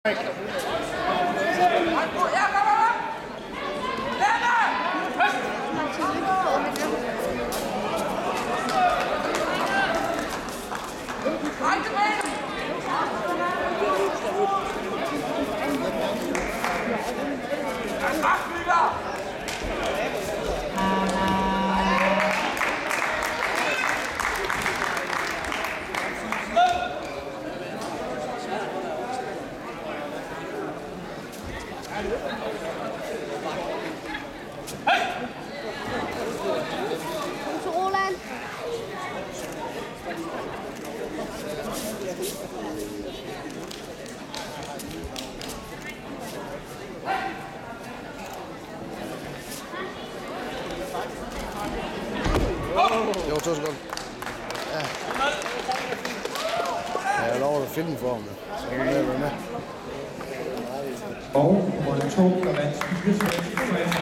Judo highlights - Danish Championships 2009 - Roland Vogt vs. Henrik Rosenberg
Judo highlights from the danish championships by Judoroland in 2009. This match is from the veterans M1 category. Here I met Henrik Rosenberg in the mens 81 kilo division, 2nd round.